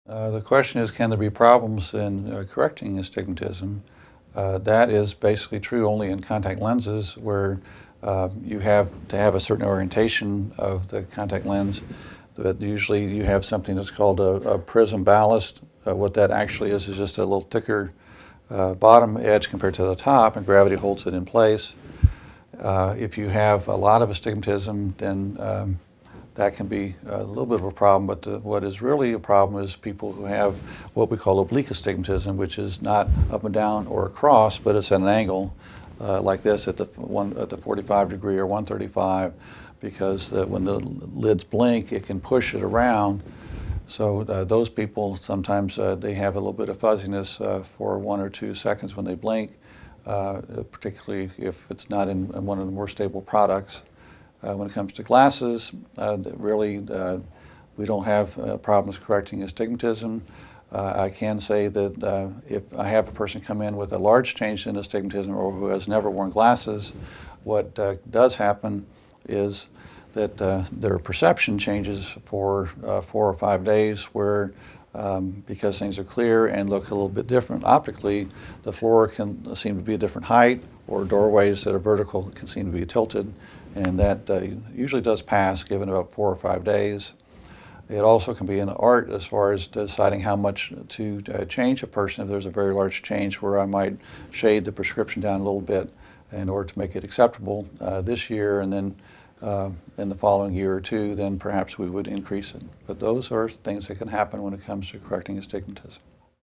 What Is Astigmatism? : Problems With Astigmatism & Lenses
When people have oblique astigmatism, which is an angled astigmatism, blurriness can occur for a few seconds when someone wears contact lenses. Discover the problems with astigmatism and lenses, and how people have to adjust to corrections, with tips from an optometrist in this free video on human eye problems and disorders.

Expert: Dr. James W. Kirkconnell
Bio: Dr. James W. Kirkconnell graduated from the University of Houston College of Optometry in 1984.